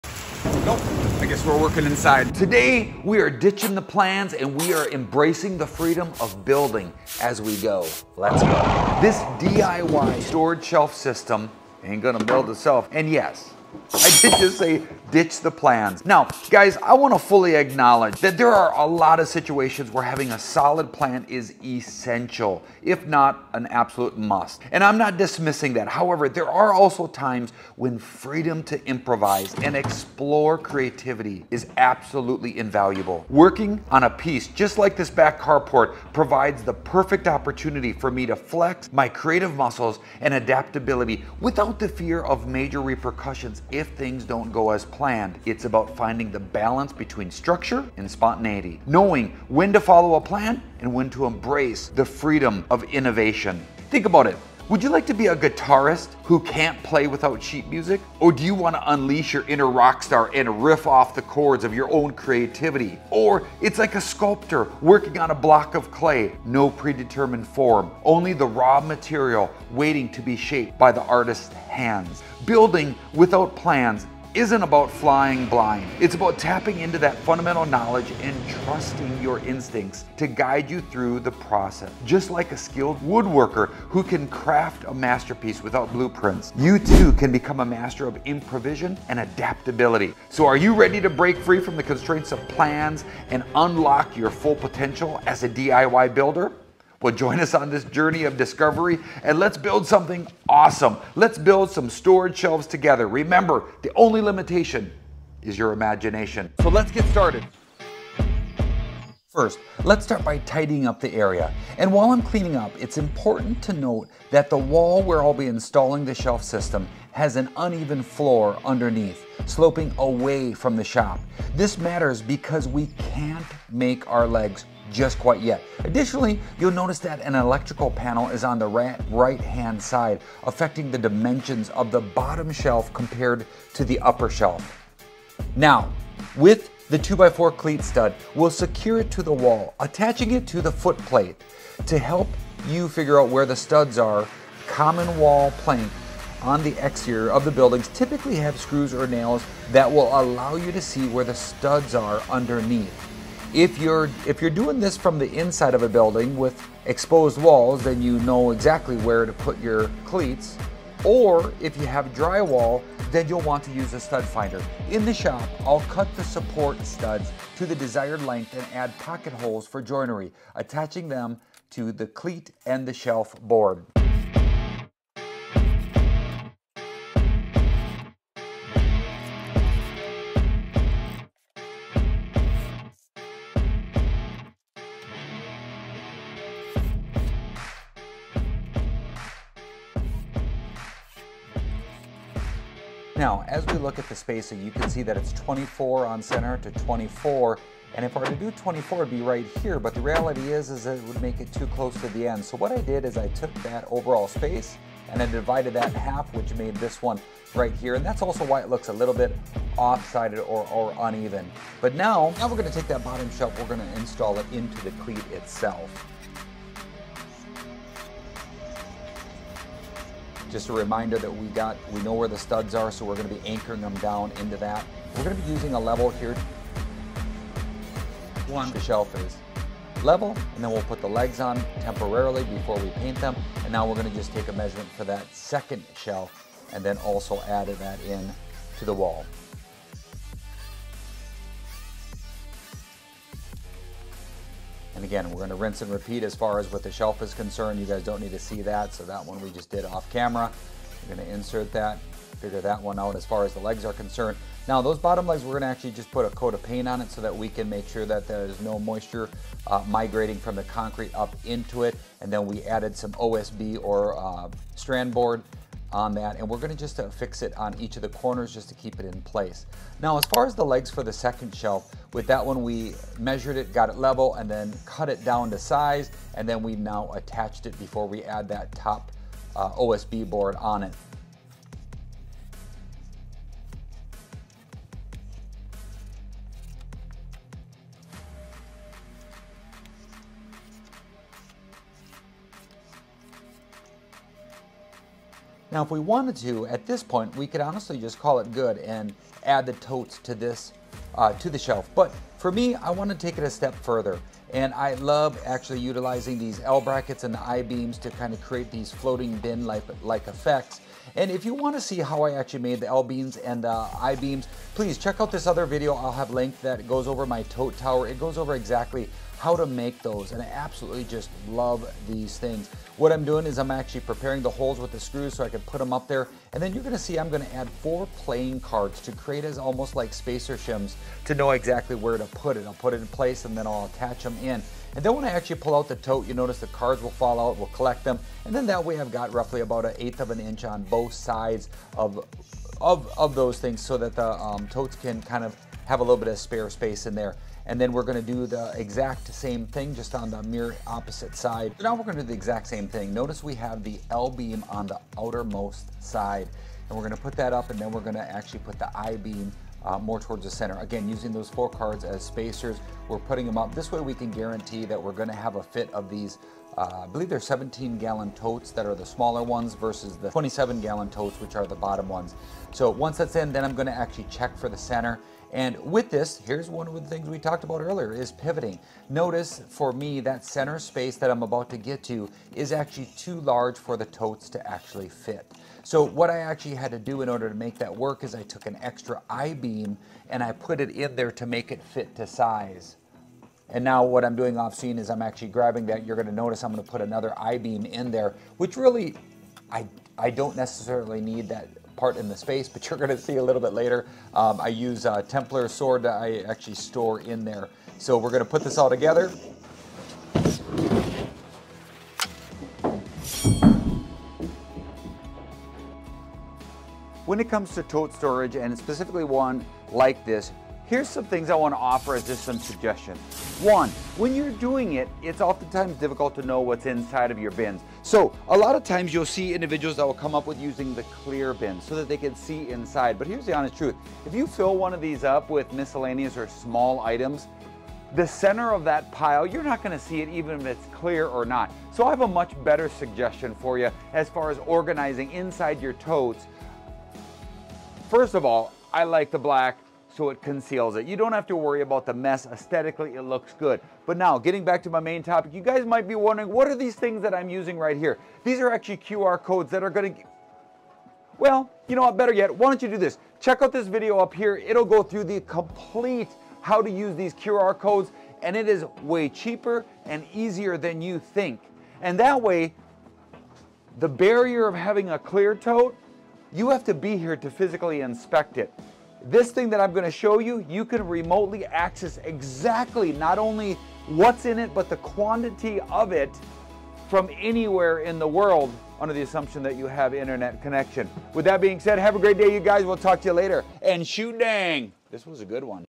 DIY Tote Storage Shelves Idea- Simple and Easy!
Are you tired of following rigid plans for your DIY projects? In this video, we challenge the conventional wisdom of meticulously planning every step and instead embrace the freedom of building as we go. Join us as we explore the benefits of spontaneity, creativity, and adaptability in DIY storage shelf construction. Say goodbye to the limitations of preconceived plans and unlock your potential as a fundamental builder. Watch now and discover how to unleash your creativity while mastering the art of DIY shelf construction!

0:00 Intro
0:10 Storage Shelf Montage
0:16 Ditch the plans
1:47 The Build
2:06 The Build- Measure/Cut/Install
3:20 Framing the shelfs
5:00 Laminate and I Beams
6:57 Black vs Clear Bins
7:22 QR Codes/Tote Scan
7:55 Outro